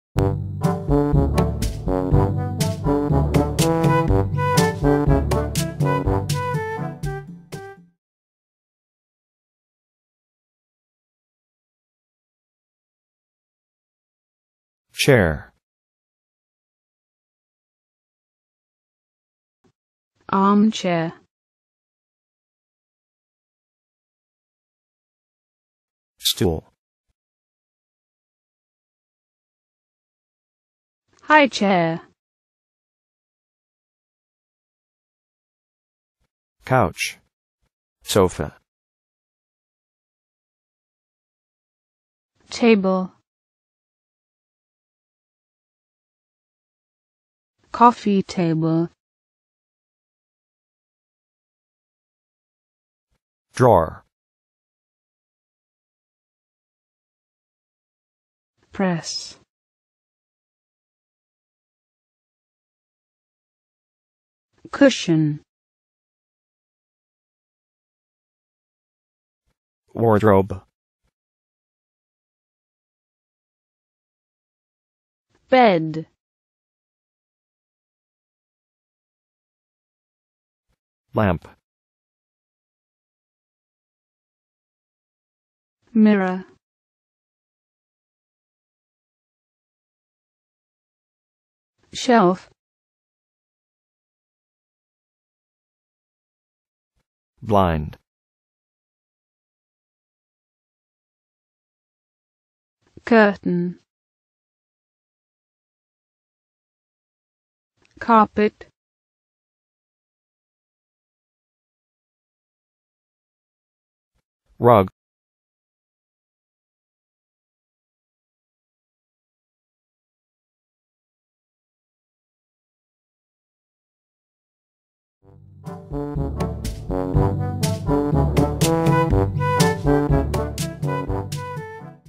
✍ Simple English - FURNITURE + Exercises
English video lesson that covers the FURNITURE vocabulary + exercises.